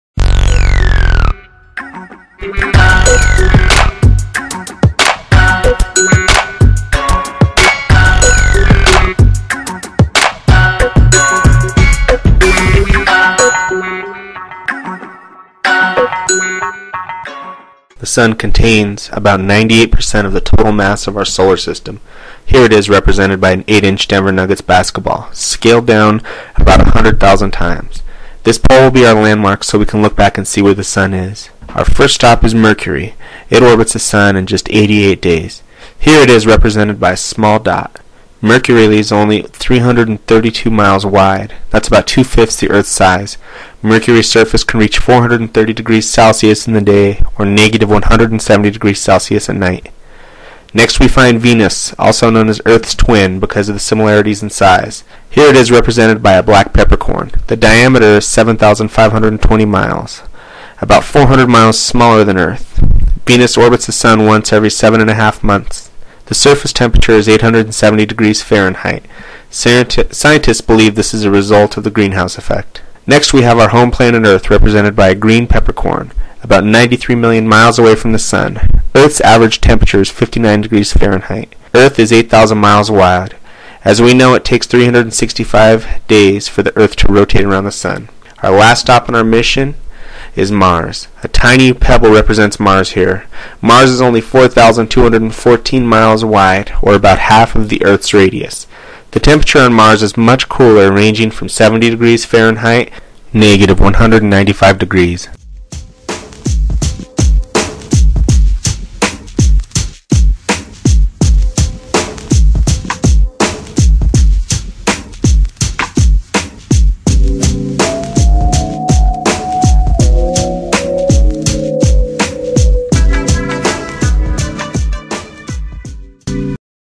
The cosmic distance scale Part 1: The terrestrial planets
This is a scale of our solar system scaled down about 100,000 times.  The sun is represented by an 8 inch Denver Nuggets basketball.  All of the planets are scaled down about 100,000 times.  This was filmed in Washington Park in Denver Colorado.